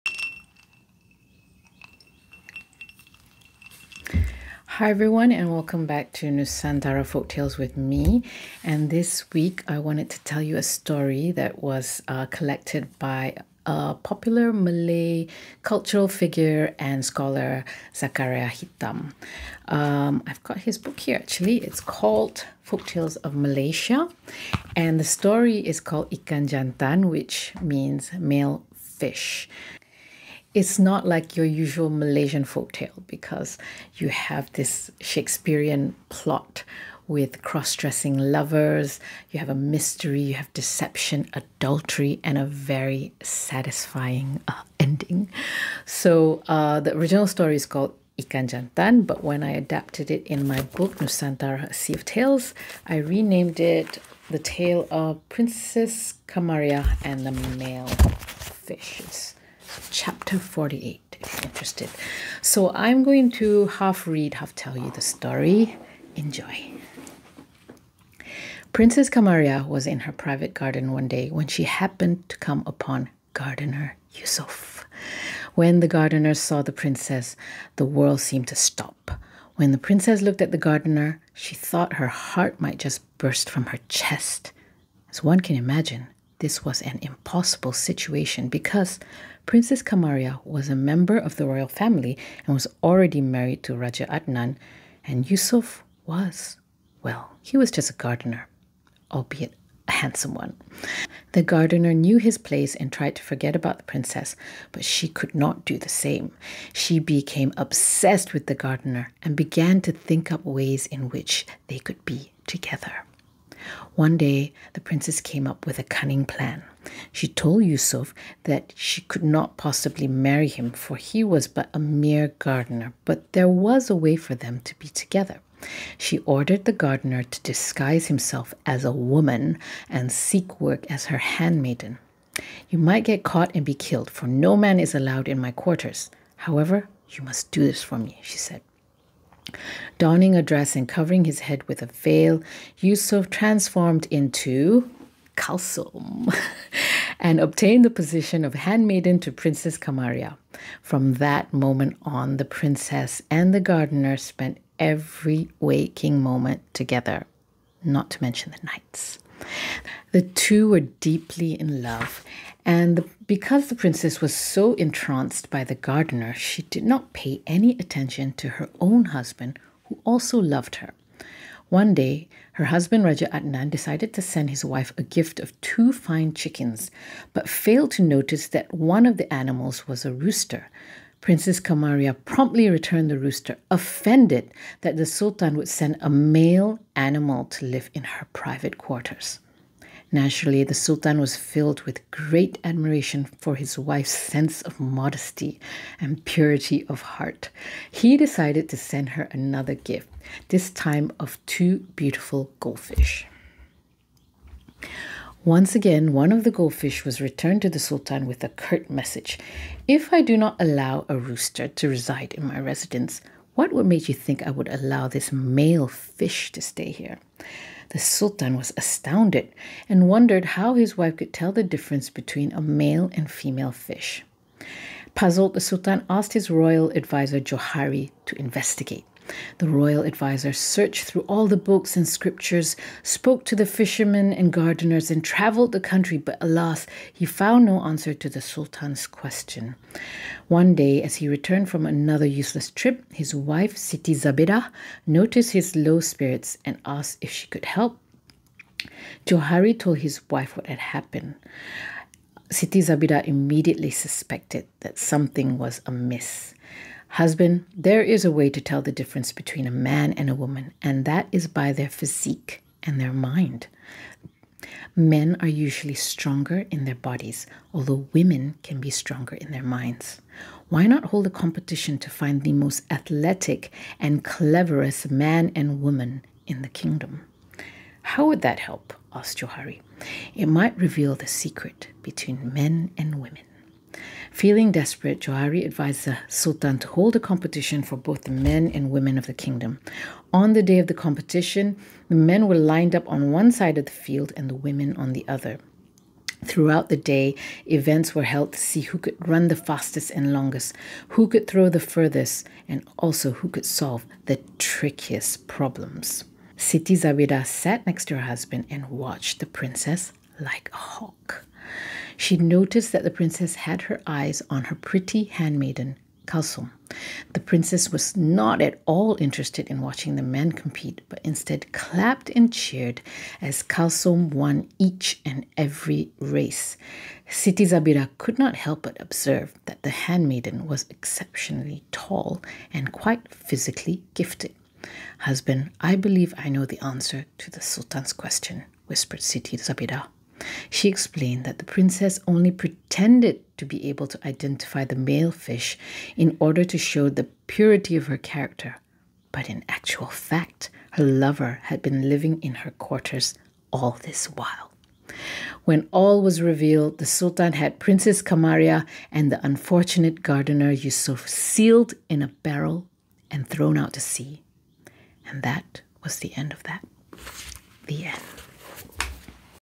PRINCESS KHAMARIAH AND THE MALE FISH//NUSANTARA FOLKTALES WITH HEIDI
In which I tell you the story of Princess Khamariah and the Male Fish